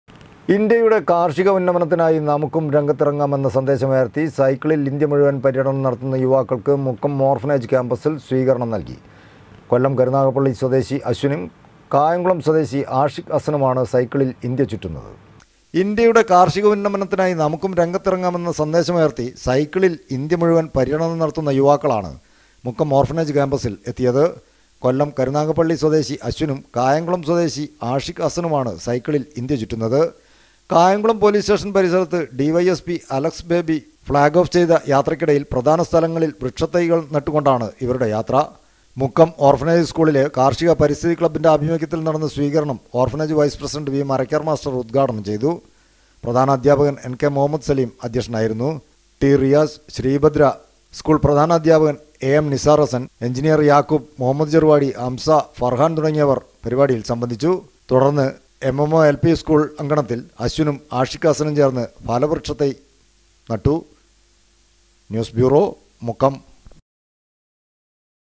ഇന്ത്യമുഴുവൻ പര്യടനം നടത്തുന്ന യുവാക്കൾക്ക് മുക്കം ഓർഫനേജ് ക്യാമ്പസിൽ സ്വീകരണം നൽകി .
ഇന്ത്യയുടെ കാർഷിക ഉന്നമനത്തിനായി നമുക്കും രംഗത്തിറങ്ങാം എന്ന സന്ദേശമുയർത്തി സൈക്കിളിൽ ഇന്ത്യമുഴുവൻ പര്യടനം നടത്തുന്ന  യുവാക്കൾക്ക്  മുക്കം ഓർഫനേജ് ക്യാമ്പസിൽ സ്വീകരണം നൽകി . കൊല്ലം കരുനാഗപ്പള്ളി സ്വദേശി അശ്വിനും കായംകുളം സ്വദേശി ആഷിക് ഹസ്സനുമാണ് സൈക്കിളിൽ ഇന്ത്യ ചുറ്റുന്നത്.